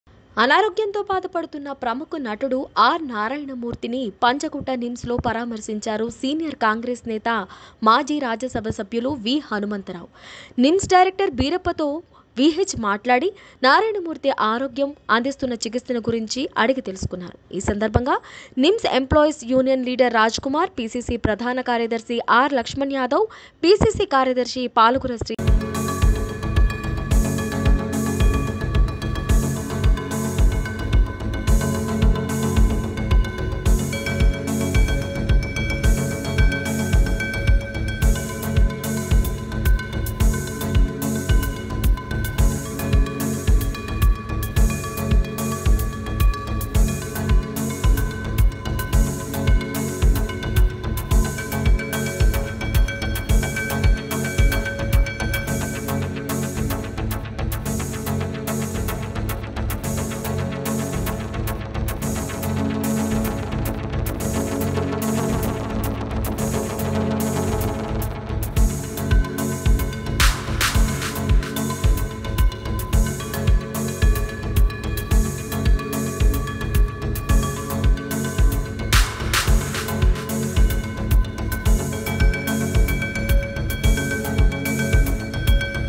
నిమ్స్ హాస్పిటల్ లో ఆర్.నారాయణమూర్తి ని పరామర్శించిన వి.హెచ్.
SITI VISION Live a News Channel dedicated to live Reports,